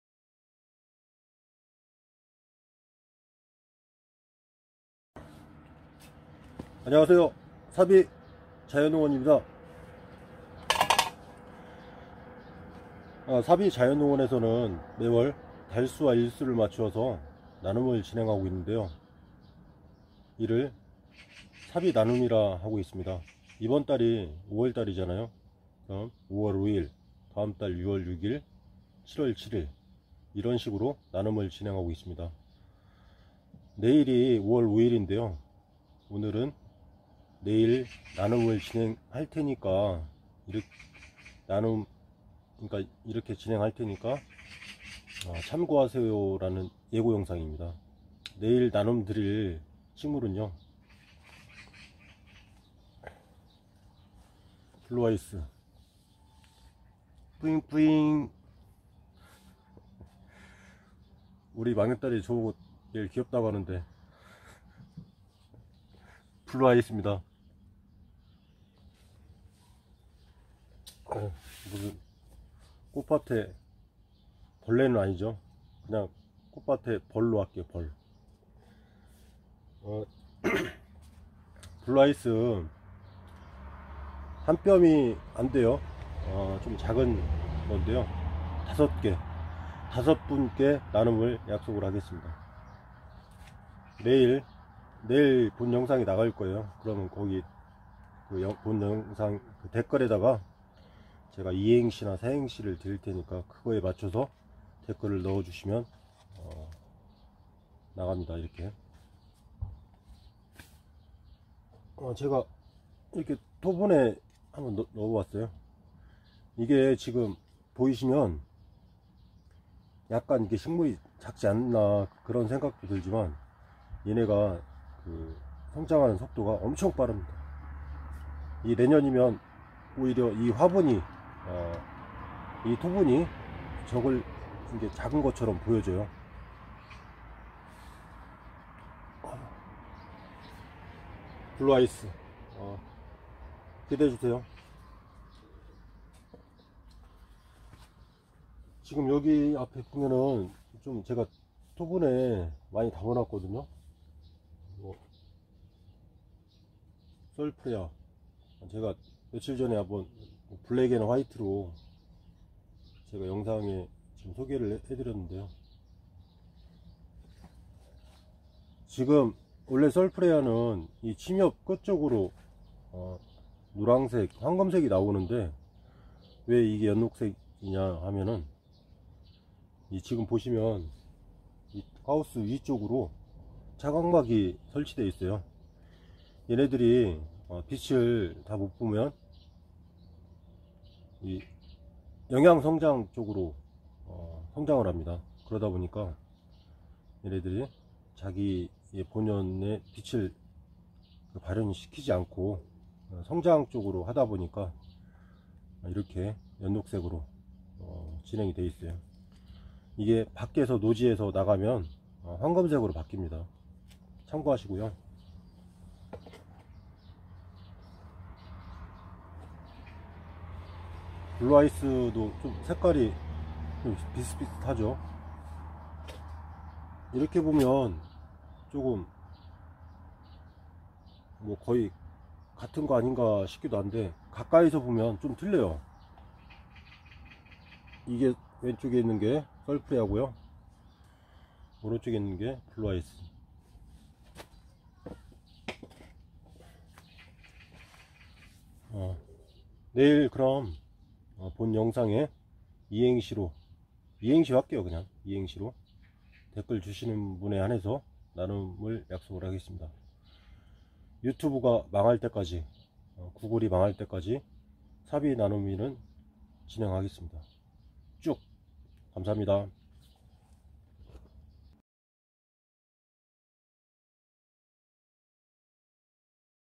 5월5일 사비나누미 나눔 이벤트 예고
사비자연농원에서는 매월 달수와 일수를 맞추어 나눔을 하고있죠 이를 사비나누미라합니다
내일 본영상 댓글에 2행시를 넣어주시는 5분께 블루아이스 묘목을 나눔드릴께요